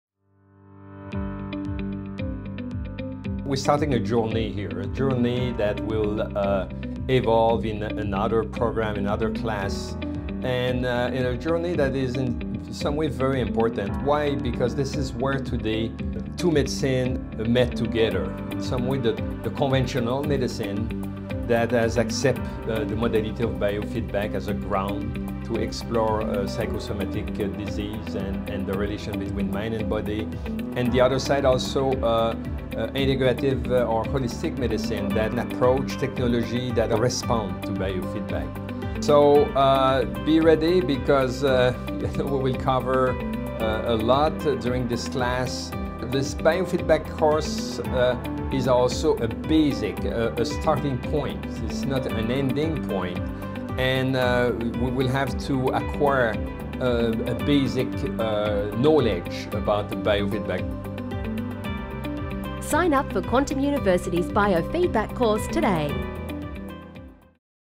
⭐ Updated Course ⭐ Biofeedback - Quantum University
This course introduces the concept of biofeedback and its relationship with consciousness in an integrative model of medicine based on the basic premises of quantum physics and increases awareness of how our unique perceptions of the world affect our internal physiology. The course summarizes common biofeedback modalities and applications and clarifies the importance of heart rate variability and its applications in restoring and balancing the autonomic nervous system in response to stress.